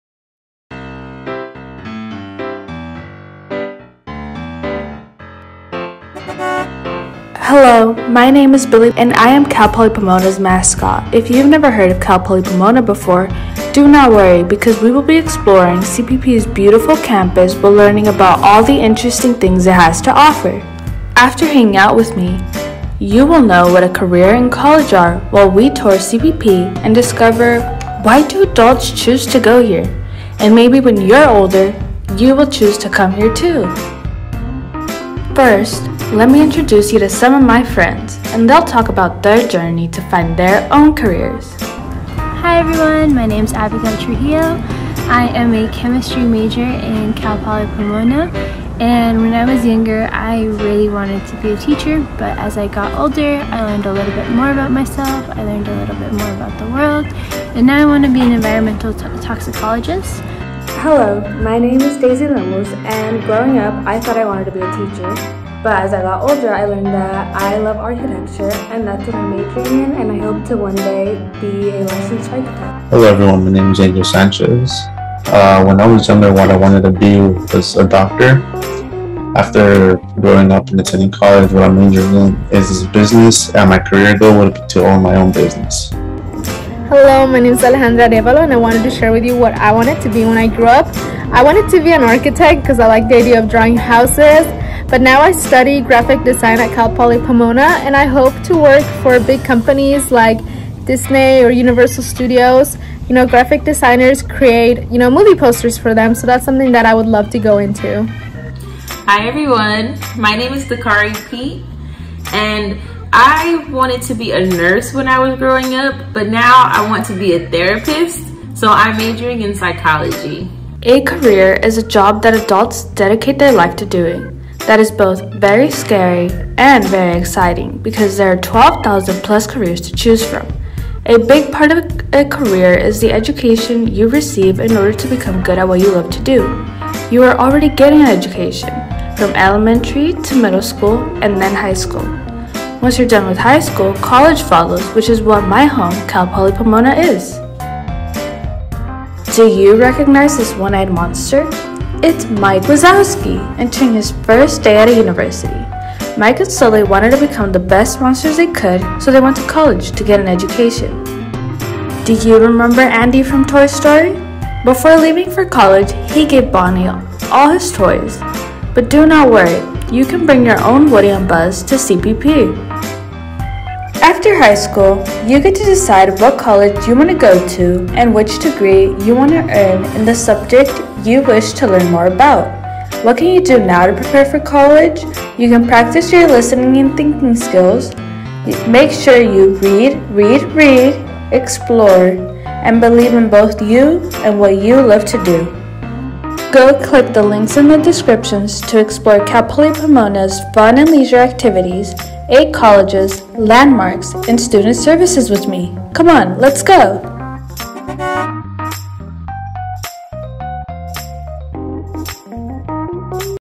Campus Exploration : Introduction
Now that we’ve discussed what a career and a major are, let's begin our trip through Cal Poly Pomona !

Chat with us below!

😊💙💚💛 Go Broncos!